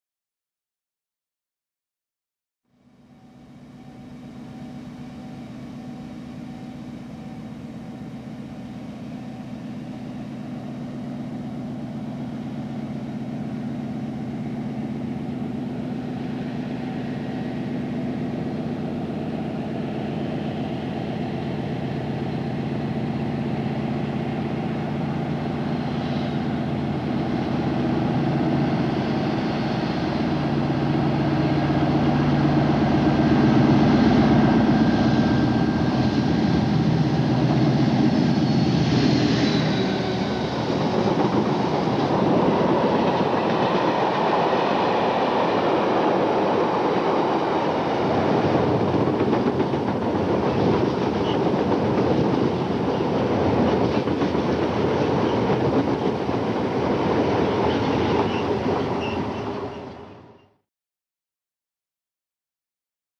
Class 56 Goods Train at Portlethen
A class 56 locomotive hauls a goods train of tankers and timber wagons through Portlethen late in the evening. Great sound as the train approaches.